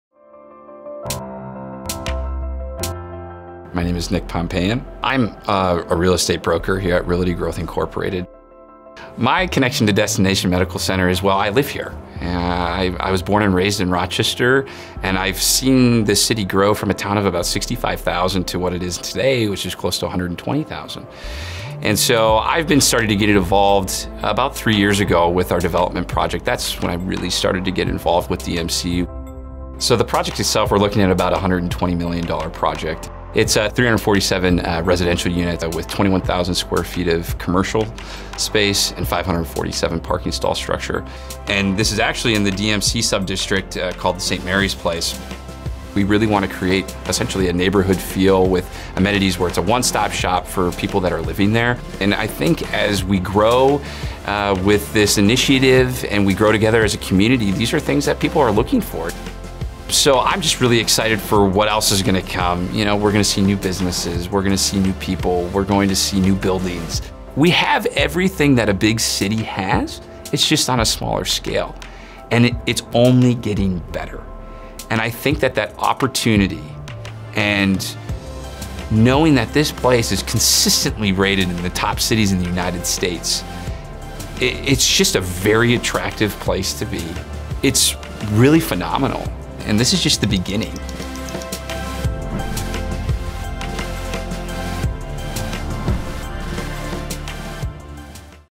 DMC Rochester, MN Storytellers Video - Growth - Nick Pompeian
Nick Pompeian, a real estate broker talks about DMC's role in developing in Rochester, Minnesota. Nick has a development project in the St Marys Place Sub-district.